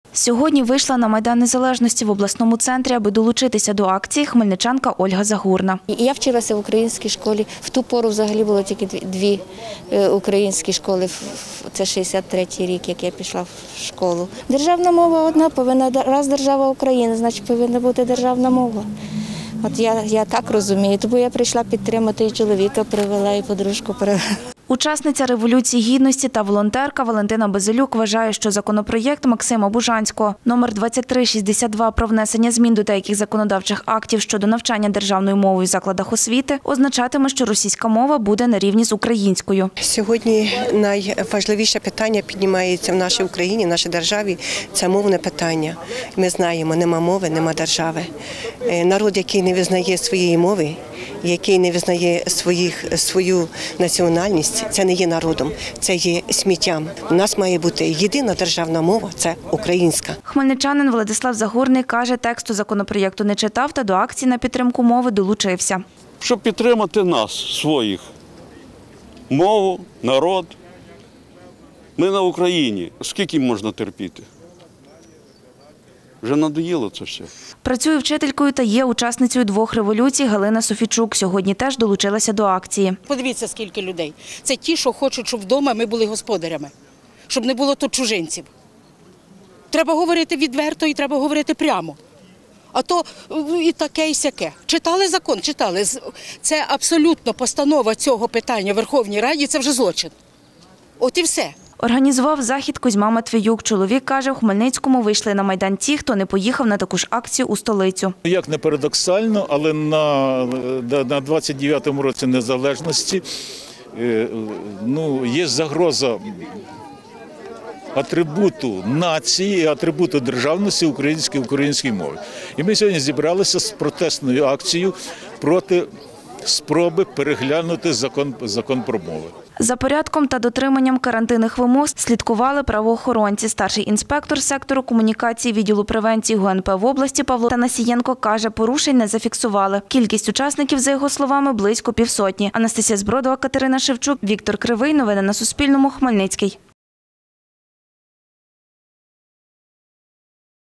«Не чіпай нашої мови»: хмельничани вийшли на акцію в підтримку української мови
На акцію, в підтримку української мови та проти законопроєкту №2362 на Майдані Незалежності в обласному центрі зібралось 70 хмельничан. Про це Суспільному сказав організатор заходу в Хмельницькому, голова товариства політв’язнів та репресованих в Хмельницькій області Кузьма Матвіюк.